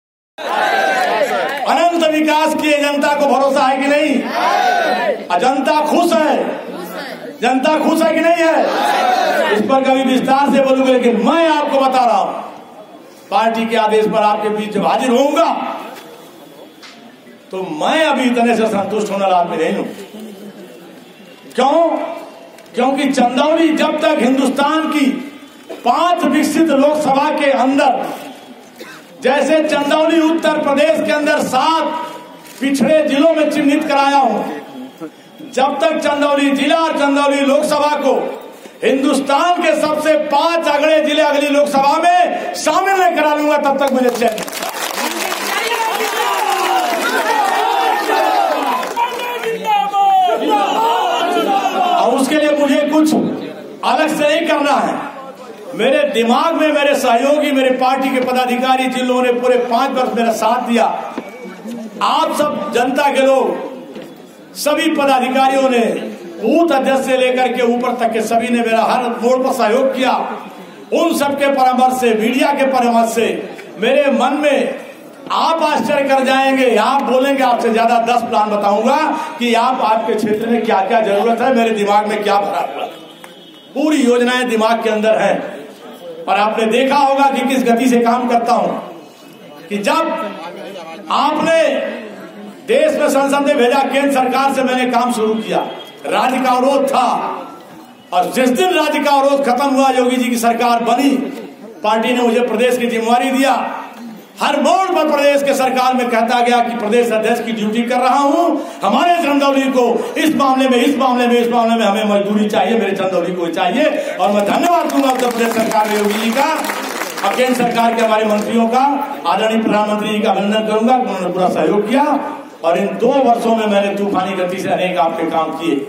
ऐसी है चंदौली के लिए सांसद महेंद्र नाथ पांडे की योजना
चंदौली जिले की मुगलसराय विधानसभा में भाजपा सांसद का जोरदार स्वागत करते लोग सांसद महेंद्र नाथ पांडे की विजय संकल्प सभा